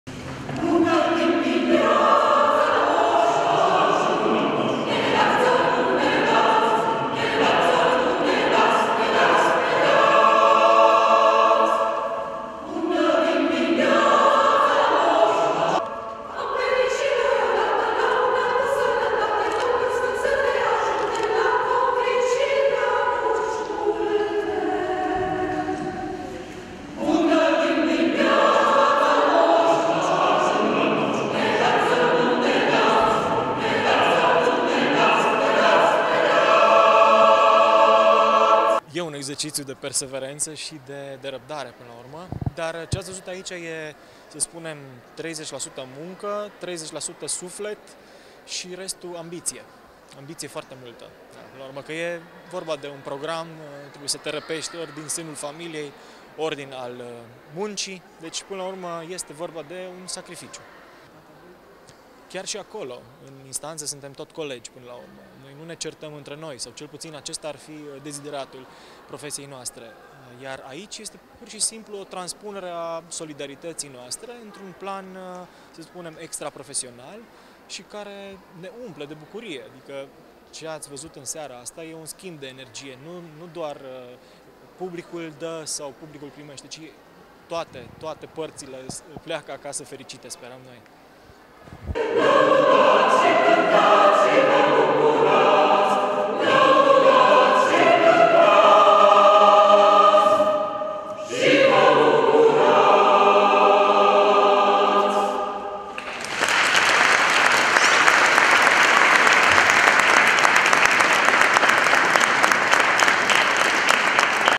Corul Avocatilor, in concert la Timisoara
Seara magica la Timisoara. Biserica plina ochi la un concert fabulos sustinut de Corul Avocatilor si mai multi copii talentati. Foto si video